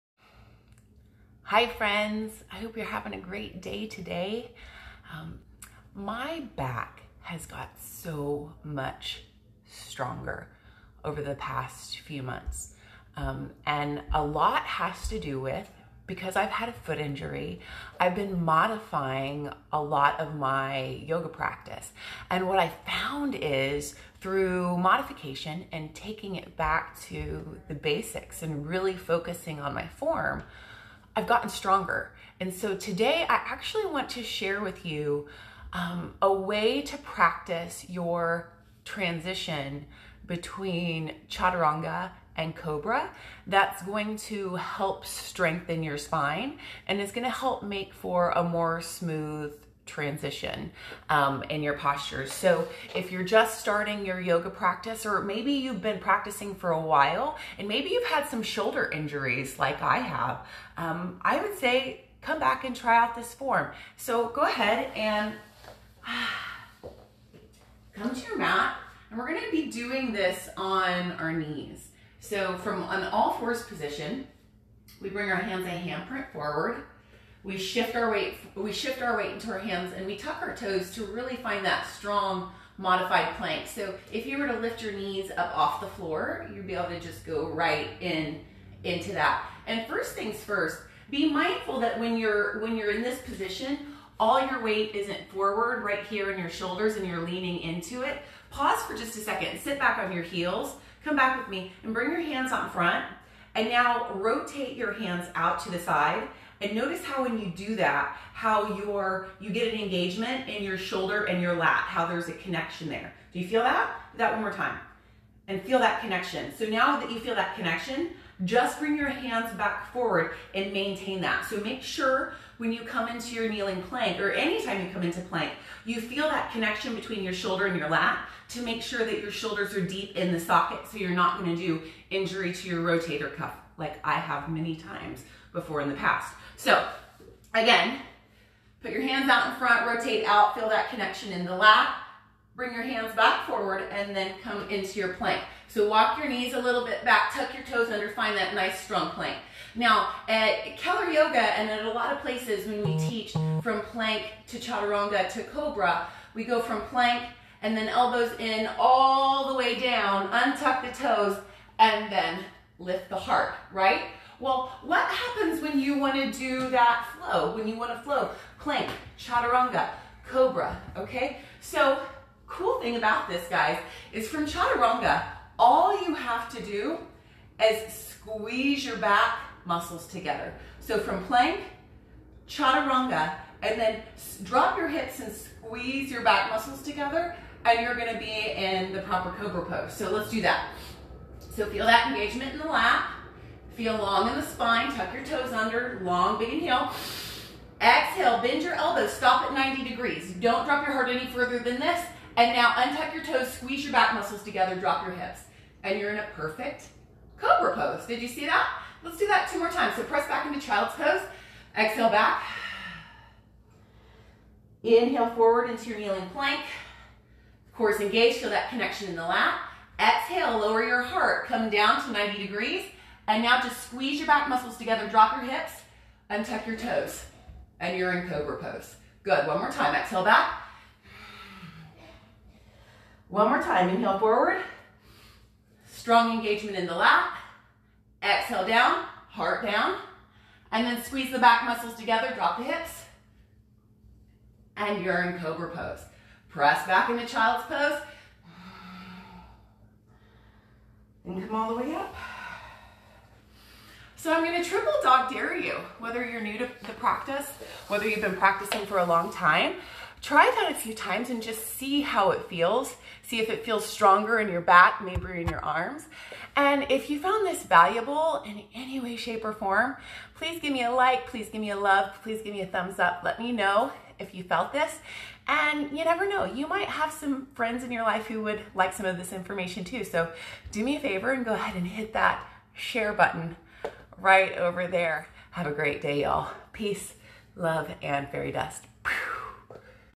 Chaturanga - Cobra Transition - Yoga Modification
To transition smoothly from Chaturanga to cobra pose, all you have to do is squeeze your shoulder blades together, drop your hips and untuck your toes.  Practice on your knees to draw your awareness into your back muscles.  sometimes modifying actually helps you get stronger, bc you can focus on your form & technique.